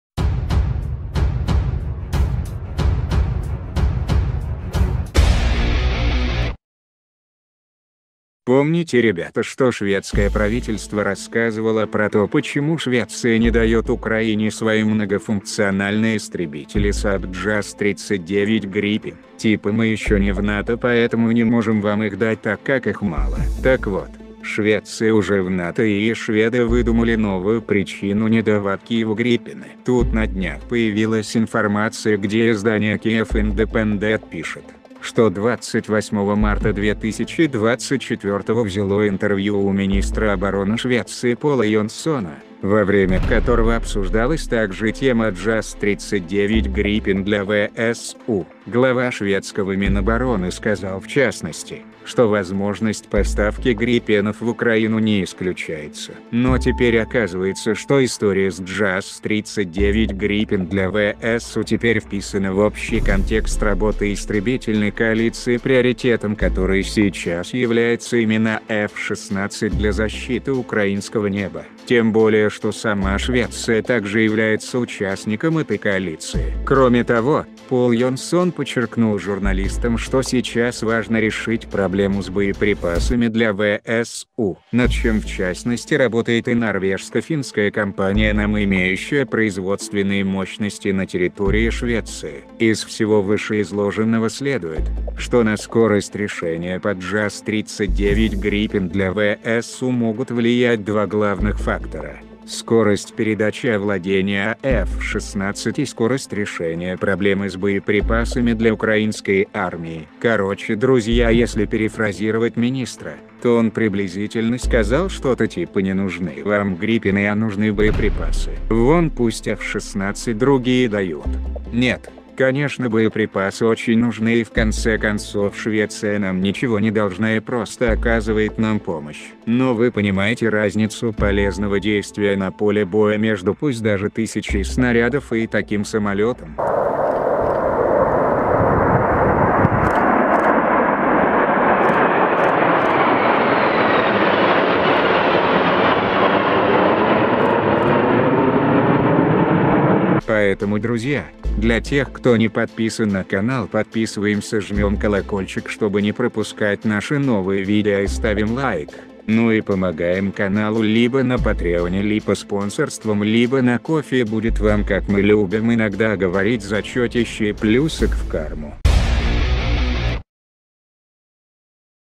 ⚡️Швеция "ЗАЖАЛА" Украине JAS 39 Gripen хотя и вступила в НАТО
История с JAS 39 Gripen для ВСУ теперь "вписана" в общий контекст работы "истребительной коалиции", приоритетом которой является именно F-16 для защиты украинского неба.